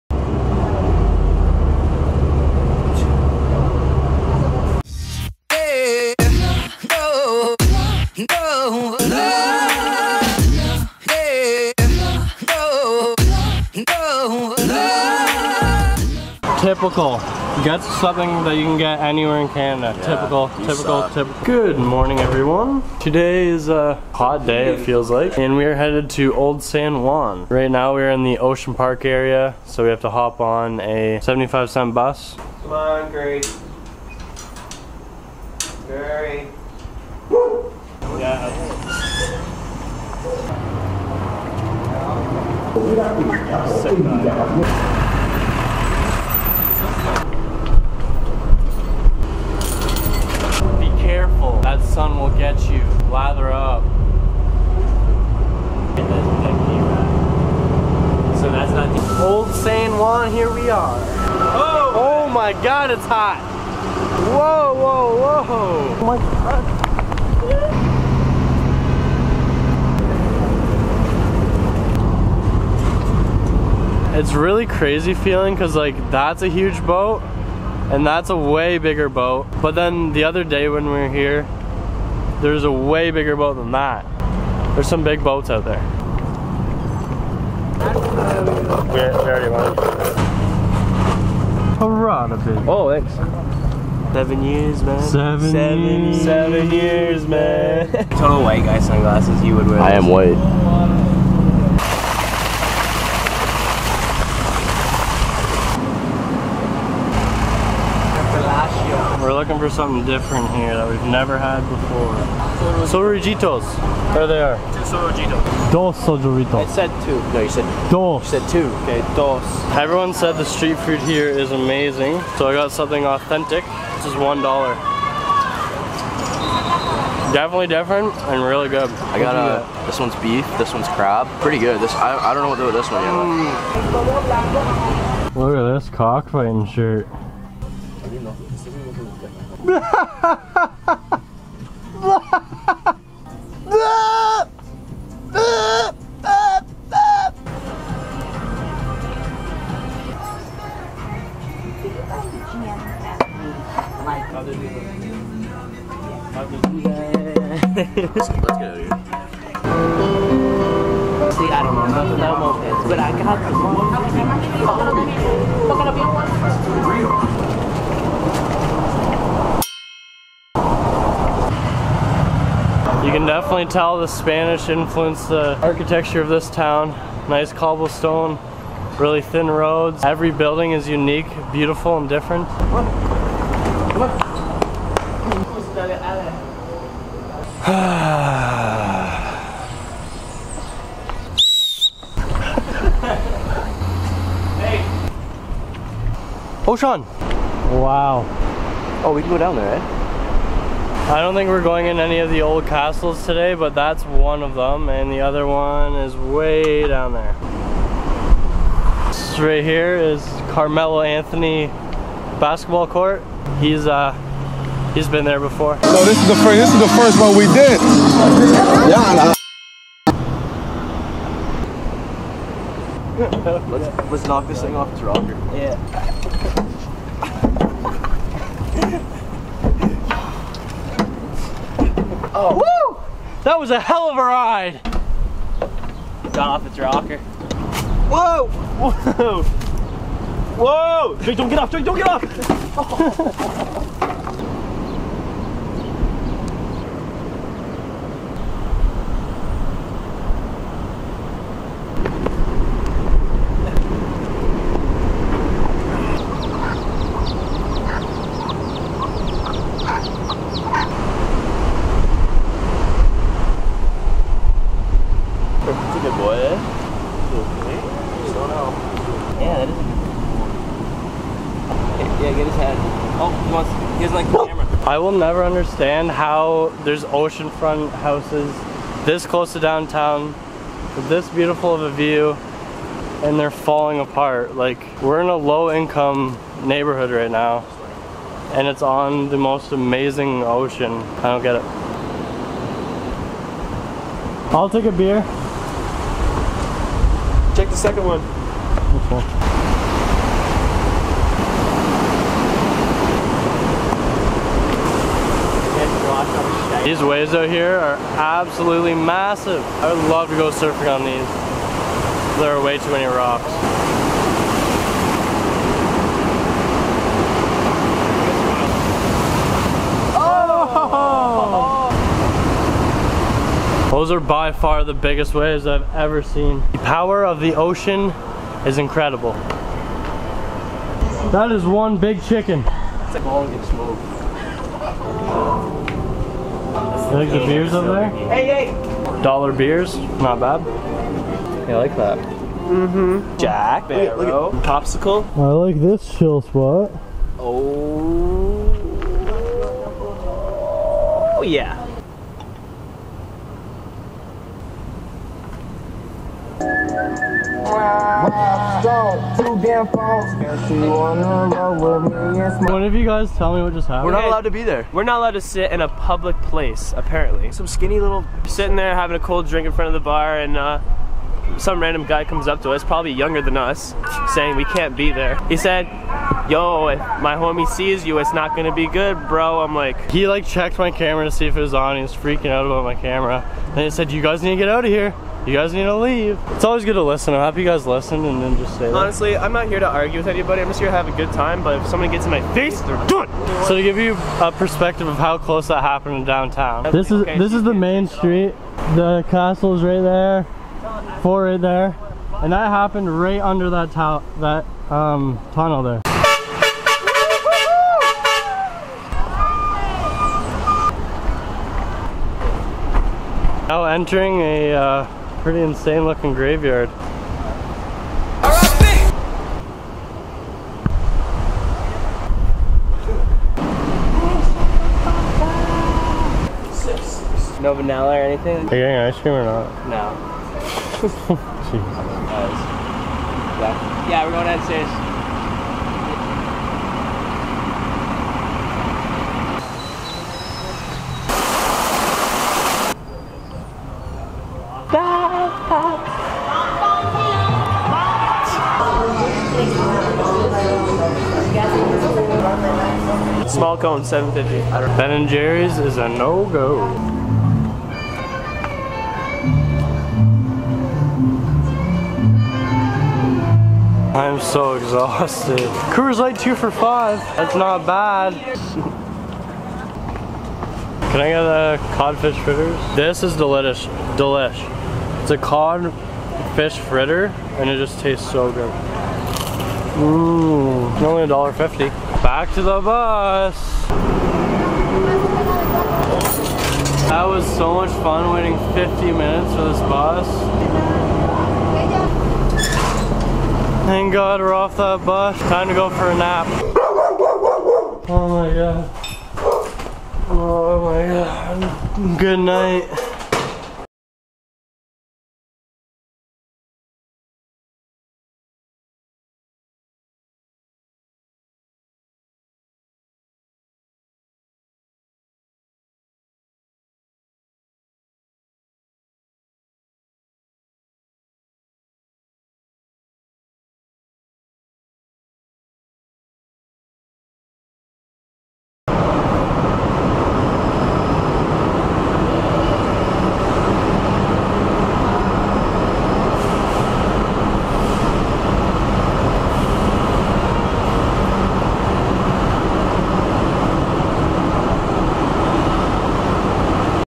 Exploring Old San Juan
The best way to plan a trip to Puerto Rico is to get a local to help you.  Check out viahero, they connect you to locals to ensure you have the best experience on their island.

Join us as we explore beautiful Old San Juan Puerto Rico as we venture off into the La Perla slums and have a little encounter.